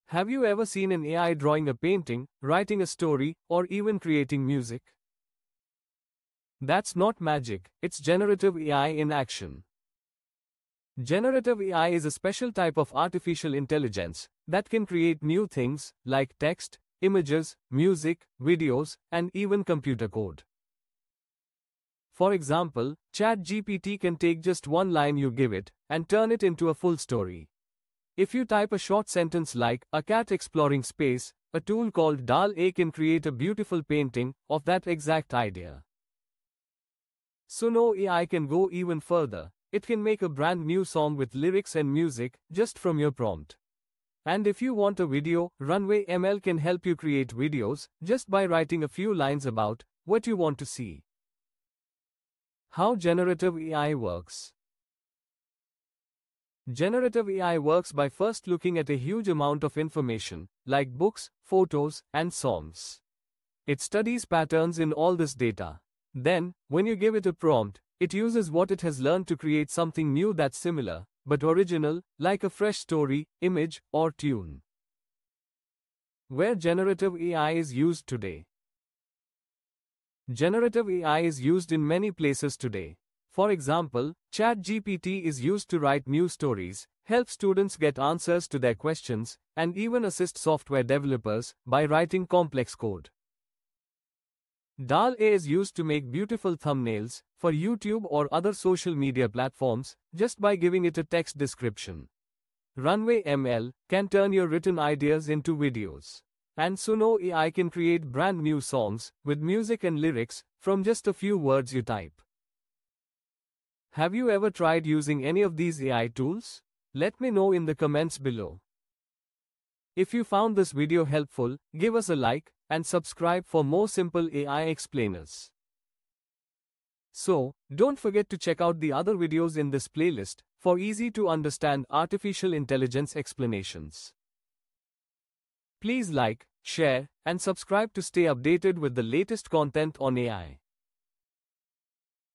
What is Generative AI? | Explained with Simple Examples
In this video we will explore  what is Generative AI.

How Generative AI Works

Where  Generative AI  is  Used  Today